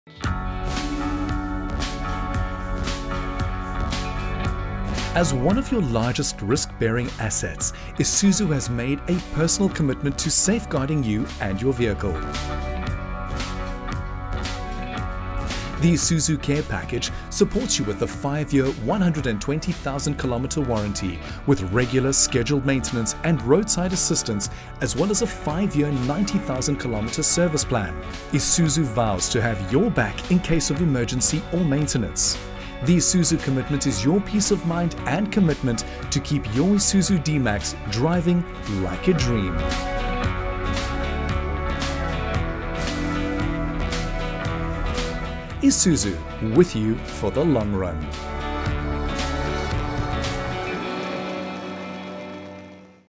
Isuzu D-Max | COMMITTED TO YOUR CONQUER
As one of your largest risk-bearing assets, Isuzu has made a personal commitment to safeguarding you and your vehicle. The Isuzu Care package, supports you with a 5 year/120 000 km warranty, with regular, scheduled maintenance and Roadside Assistance as well as a 5 Year/90 000km Service Plan. Isuzu vows to have your back in case of emergency or maintainence – the Isuzu commitment is your peace of mind and commitment to keep your Isuzu D-MAX driving like a dream.

Isuzu, with you for the long run.